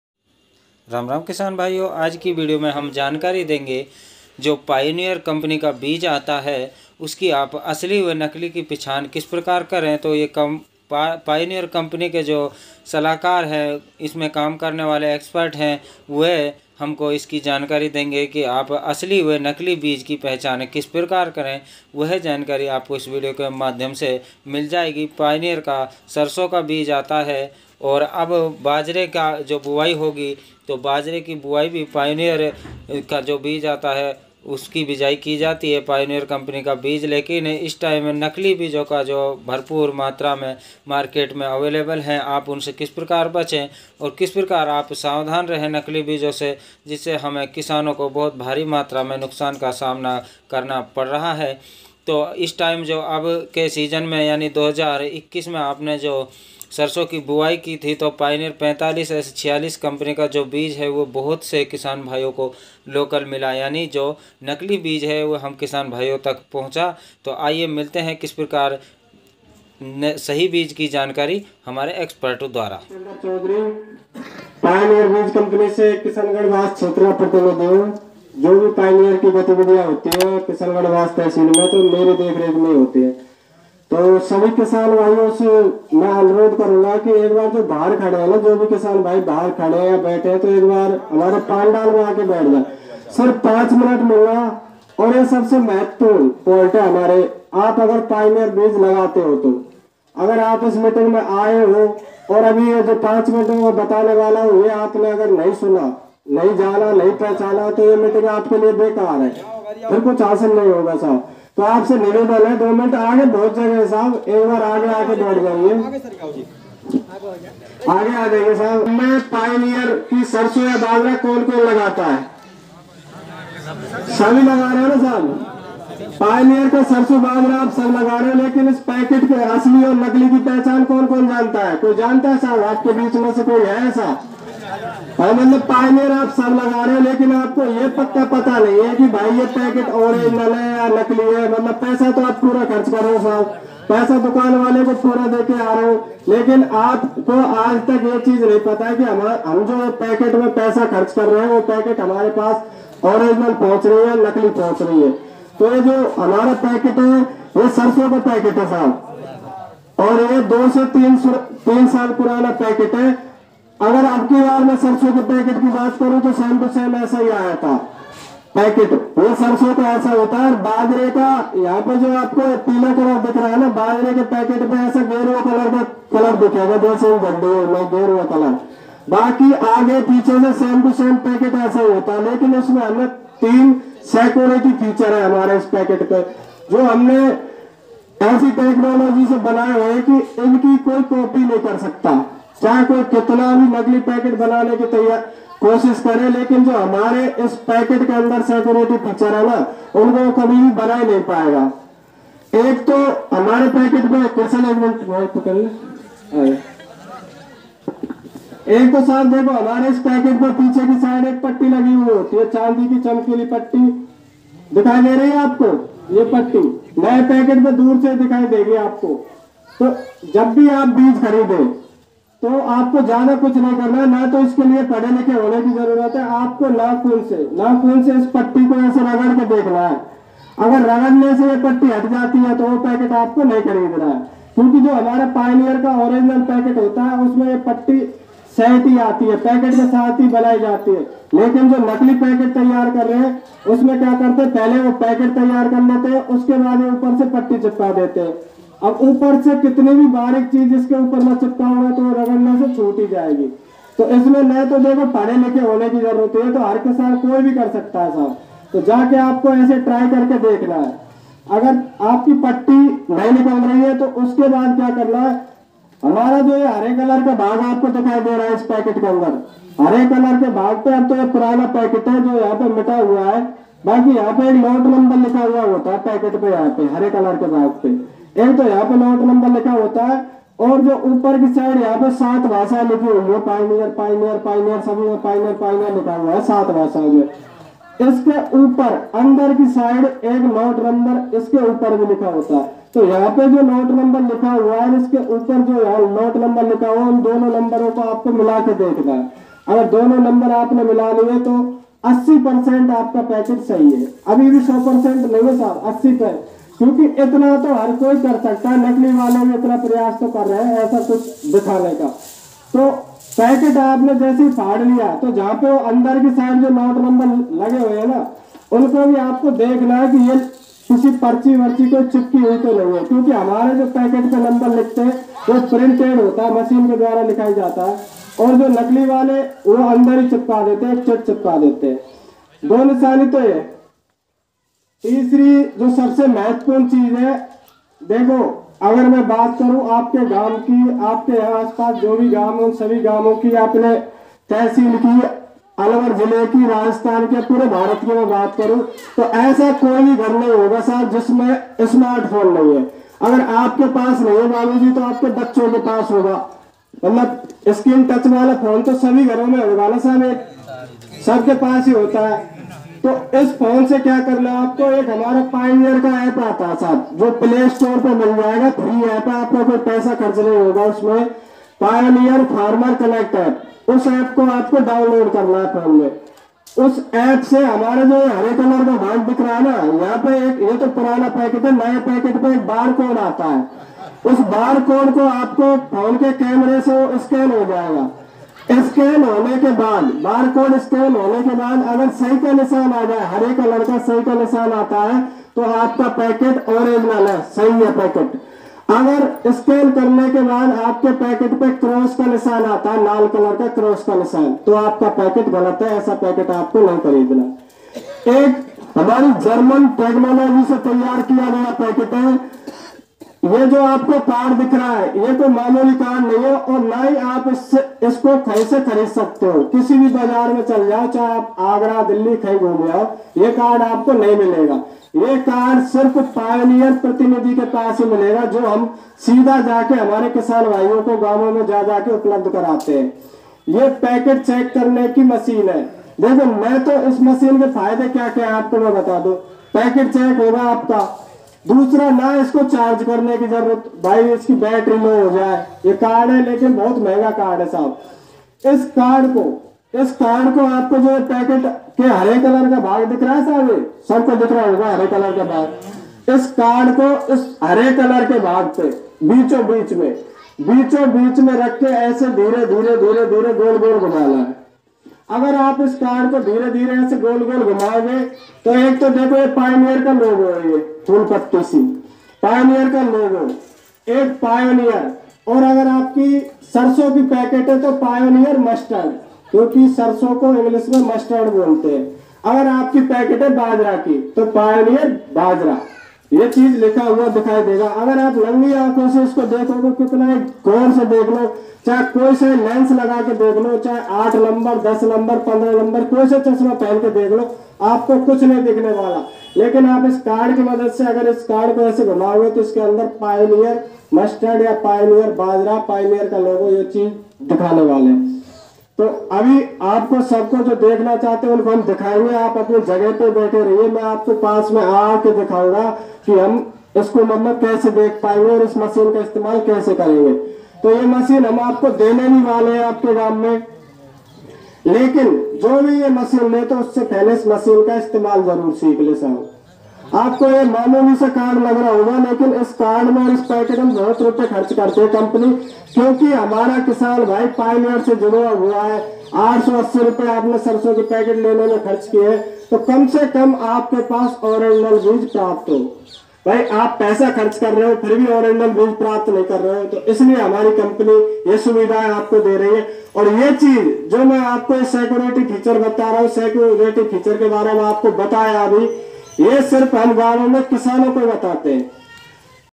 ओरिजिनल पायनियर बीज की पहचान।45S46 पायनियर बीज बाजरा व सरसों की असली की पहचान। Pioneer original seed
पायनियर बीज जो आता है सरसों या बाजरे का।
उसकी ओरिजिनल पहचान किस प्रकार आप ओरिजिनल बीज आप तक पहुंचे।
किस प्रकार आप बीज की पहचान करें।
पायनियर कंपनी के असली बीज को आप किस प्रकार पहचान।
यही जानकारी पायनियर कंपनी के जो 1 अधिकारी हैं उन द्वारा दी जा रही है।

यह संपूर्ण जानकारी आपको पायनियर कंपनी के एक्सपर्ट द्वारा दी जा रही है।

खेती-बाड़ी संबंधित वीडियो के लिंग के नीचे दी जा रही है पर क्लिक करके वीडियो देखें 👇